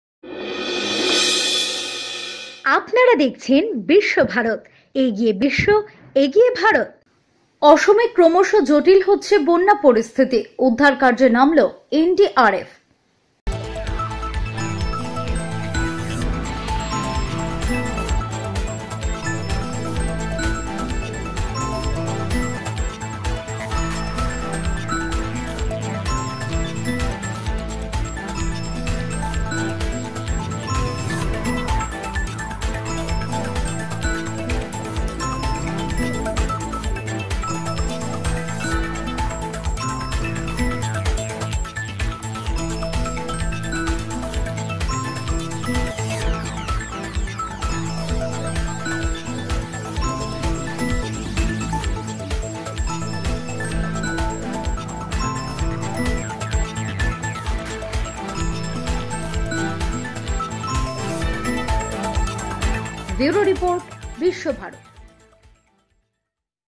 অসমে ক্রমশ জটিল হচ্ছে বন্যা পরিস্থিতি, উদ্ধারকার্যে নামলো এনডিআরএফ#biswabharat#bbharat
অসমে ক্রমশ জটিল হচ্ছে বন্যা পরিস্থিতি, উদ্ধারকার্যে নামলো এনডিআরএফ#Flood#